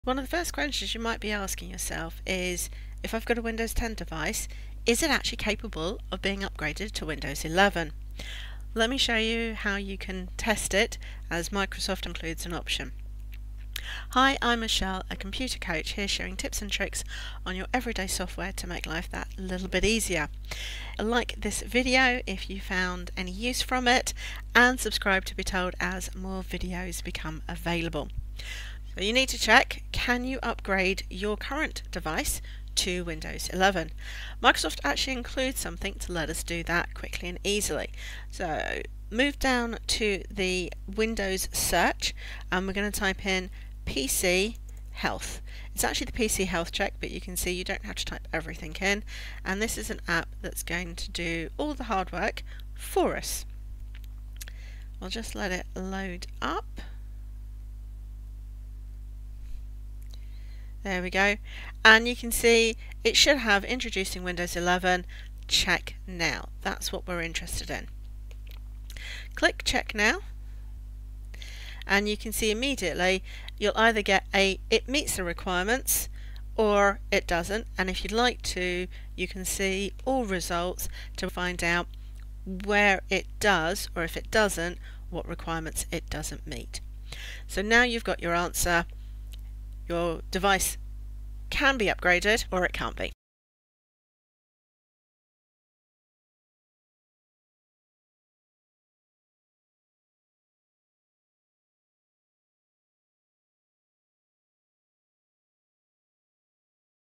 Will Your Computer Run Windows 11? Check Compatibility in 60 Seconds
In this video, I show you how to quickly check if your Windows 10 device meets the requirements for upgrading to Windows 11 using Microsoft's built-in PC Health Check app. No need for complicated third-party software or technical knowledge!

I guide you through the simple process of finding and running the PC Health Check tool that will instantly tell you if your computer is compatible with Windows 11. The app gives you a clear "yes" or "no" answer and provides details about which specific requirements your device meets or doesn't meet.

Whether you're planning to upgrade soon or just curious about your options, this quick tutorial will give you the information you need in just a few minutes.

I'm Michelle, your computer coach, sharing simple solutions to common tech challenges.

00:00 Will Your Computer Run Windows 11? Check Compatibility in 60 Seconds
00:14 Welcome Intro
00:28 PC Health Check